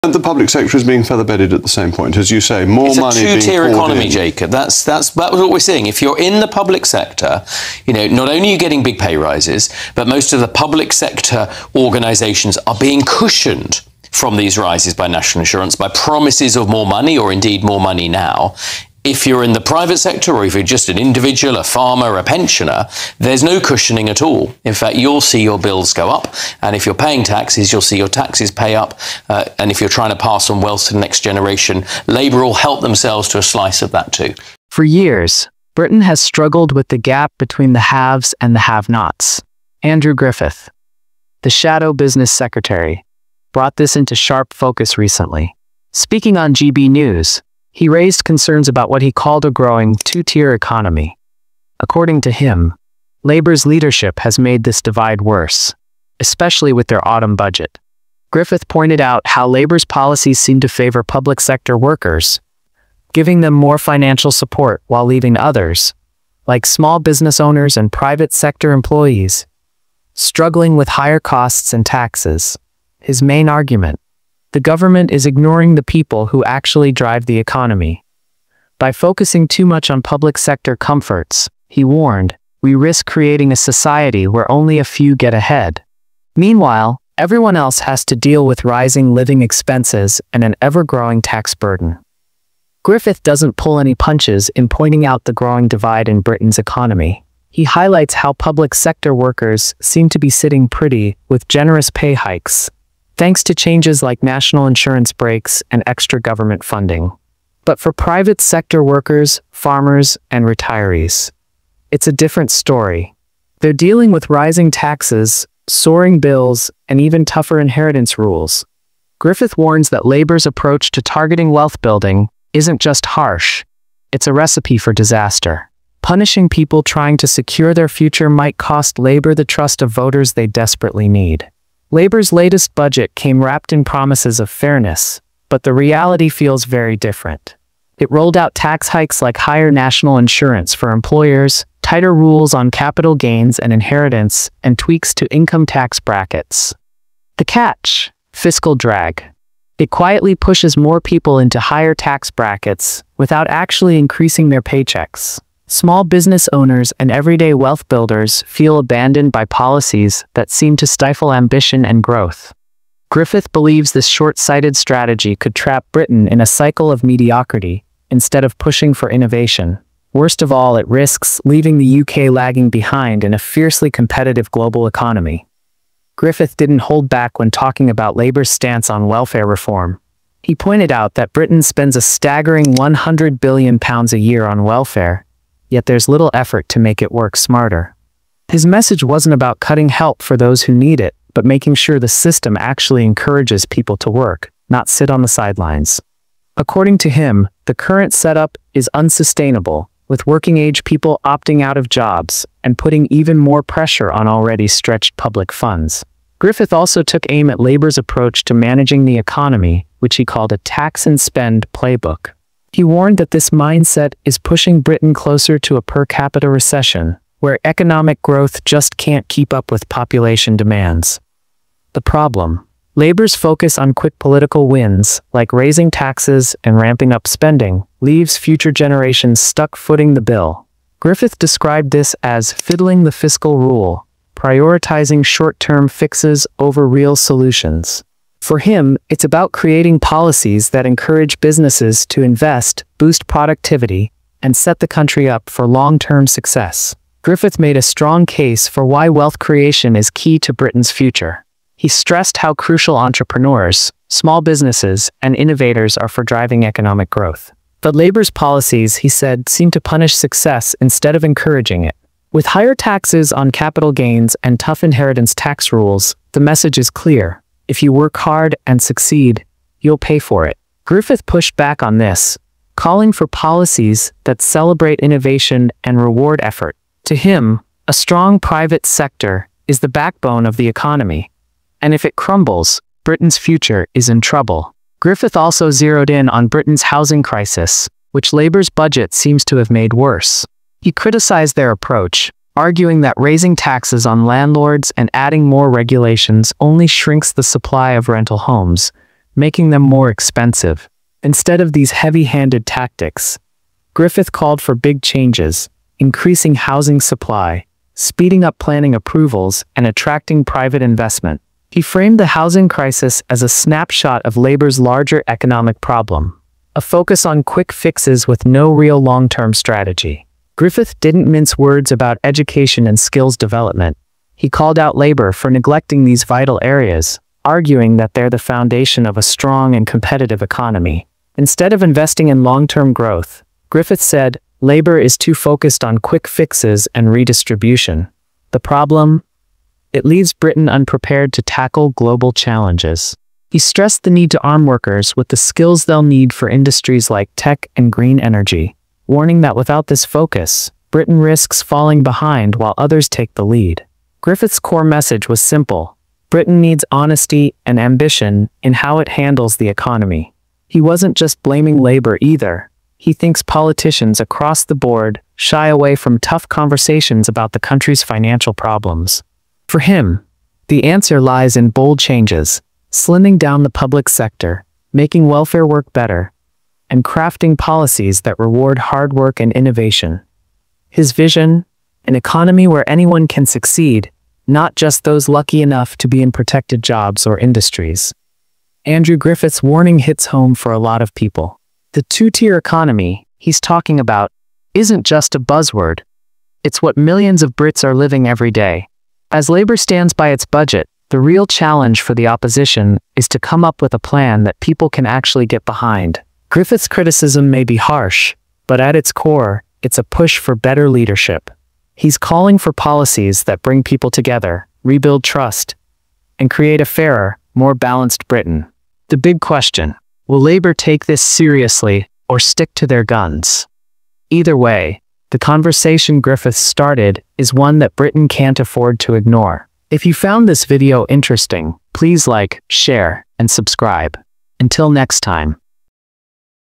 Andrew Griffith DESTROYS Keir Starmer With THIS Warning!
🔴 BREAKING: Andrew Griffith Issues Stark Warning to Labour - "It’s a Two-Tier Economy!"

Andrew Griffith, the Shadow Business Secretary, didn’t hold back when criticizing Labour's Autumn Budget. He painted a grim picture of the future under Labour’s economic plans, highlighting concerns about rising taxes and the creation of what he called a “two-tier economy.” According to him, these policies could lead to a sharp divide between the public and private sectors, potentially deepening economic inequality and setting the stage for broader issues. Griffith’s critique seems aimed at stirring debate about whether Labour’s approach is truly in the nation’s best interest or if it risks alienating key sectors of the economy.